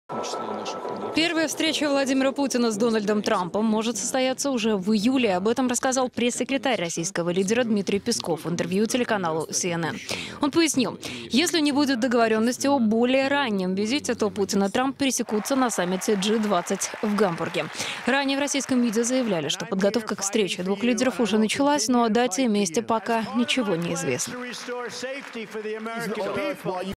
В Кремле заявили, что Путин и Трамп могут встретиться на саммите G20 в июле
Песков: Саммит в Гамбурге - первое место, где могут встретиться главы России и США